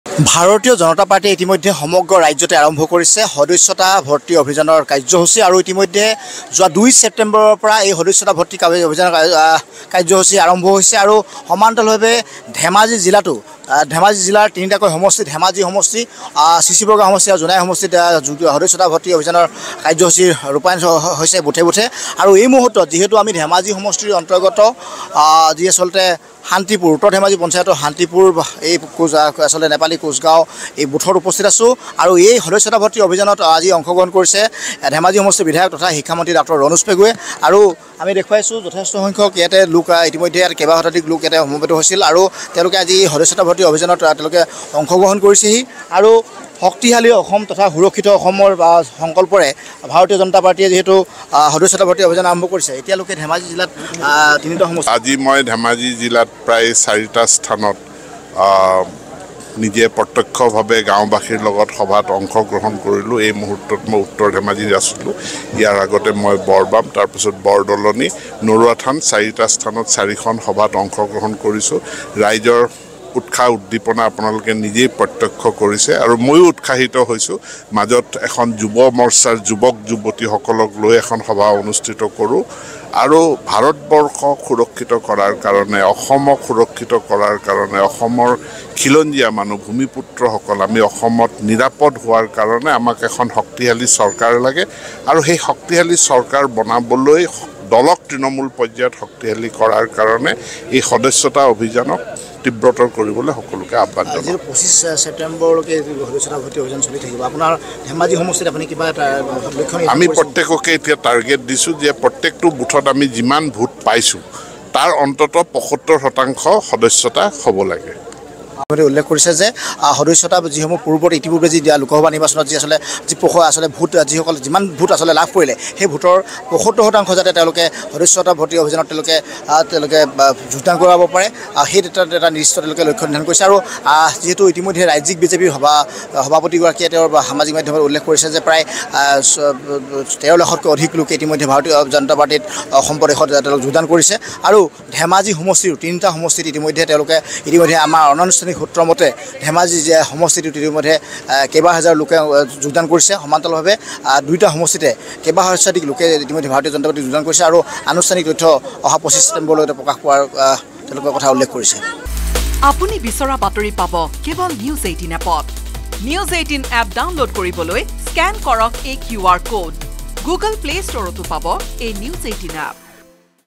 BJP's Membership Campaign at Dhemaji | ধেমাজিৰ তিনিটাকৈ বিধানসভা সমষ্টিৰ বুথত বিজেপি ভৰ্তিকৰণ। N18V
BJP's Membership Campaign at Dhemaji | লোকসভা নিৰ্বাচনৰ পিছতেই বিজেপি পুনৰ নামি পৰিছে সাংগঠনিক তৎপৰতাত
বিজেপিৰ সদস্যতা ভৰ্তি অভিযানৰ অংশ হিচাপে ধেমাজিতো তিনিটাকৈ বিধানসভা সমষ্টিৰ বুথত চলিছে এই কাৰ্যসূচী
মন্ত্ৰী ডা° ৰনোজ পেগুৱে এটা দিনতে অংশগ্ৰহণ কৰিছে চাৰিখনকৈ সভাত

লগতে চাওঁক দিনটোৰ বহু গুৰুত্বপূৰ্ণ খবৰ |